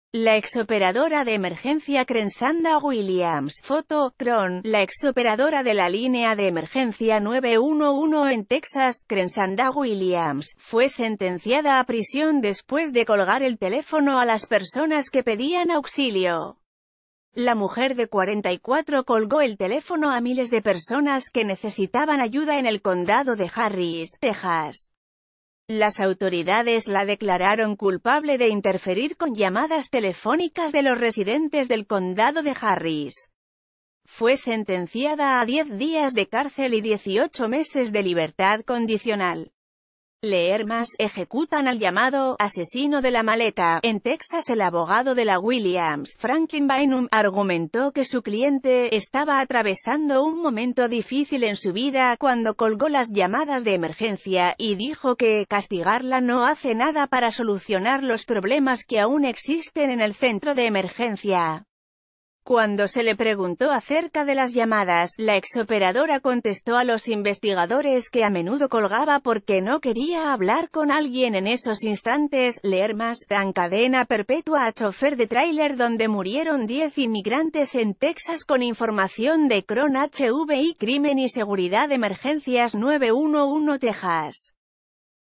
Encarcelan a operadora de emergencias 911 por colgar las llamadas
La exoperadora de emergencia Crenshanda Williams (Foto: chron) La exoperadora de la línea de emergencia 911 en Texas, Crenshanda Williams, fue sentenciada a prisión después de colgar el teléfono a las personas que pedían auxilio. La mujer de 44 colgó el teléfono a miles de personas que necesitaban a...